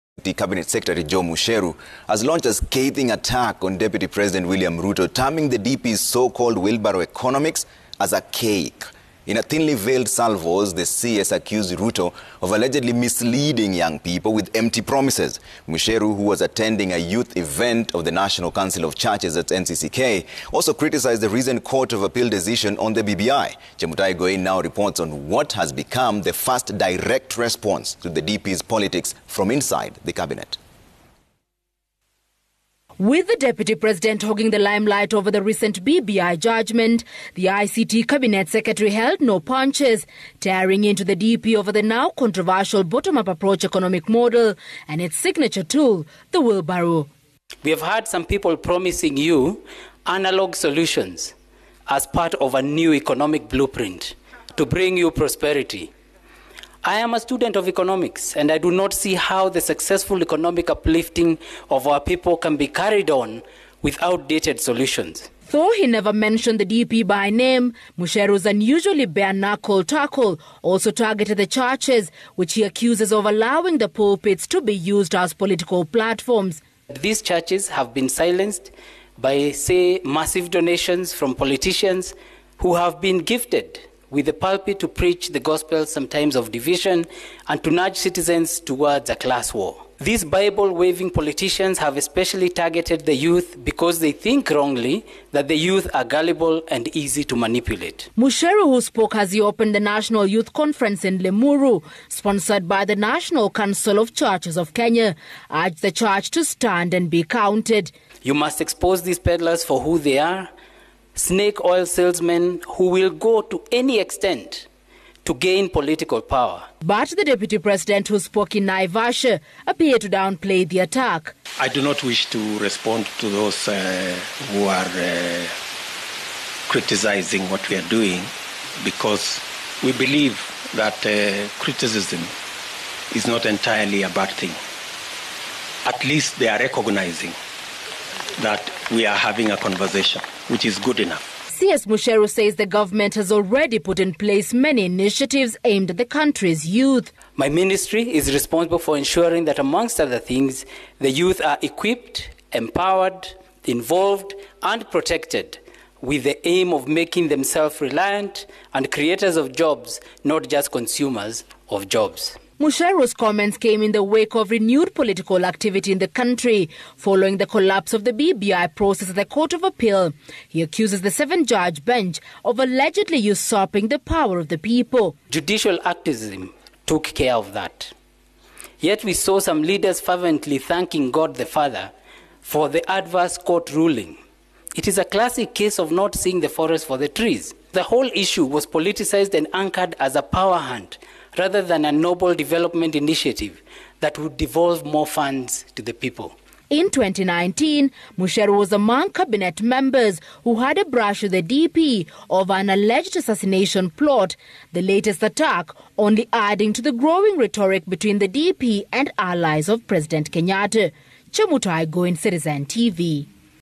ICT CS launches scathing attack on DP Ruto over promises to youth
ICT Cabinet Secretary Joe Mucheru has launched a scathing attack on Deputy President William Ruto, terming the DP’s so-called wheelbarrow economics as archaic. In thinly veiled salvos, the CS accused Ruto of allegedly misleading young people with empty promises. Mucheru who was attending a youth event of the national council of churches (NCCK), also criticized the recent court of appeal decision on the BBI. Chemutai Goin now reports on what has become the first direct response to the DP’s politics from inside the cabinet.